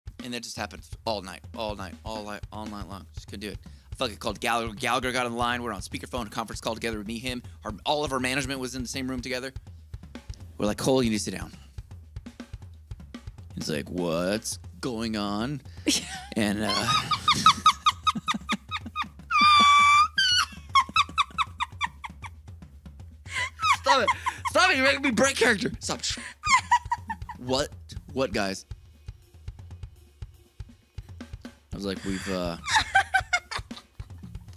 Brad turns Addie into a seagull with 1 Cole Rolland hair flip
Next Stream Dates on TWITCH (usually): 🎵MONDAY 4pm PT  🎵WEDNESDAY 4pm PT 🎵FRIDAY 1pm PT

Discord.gg/halocene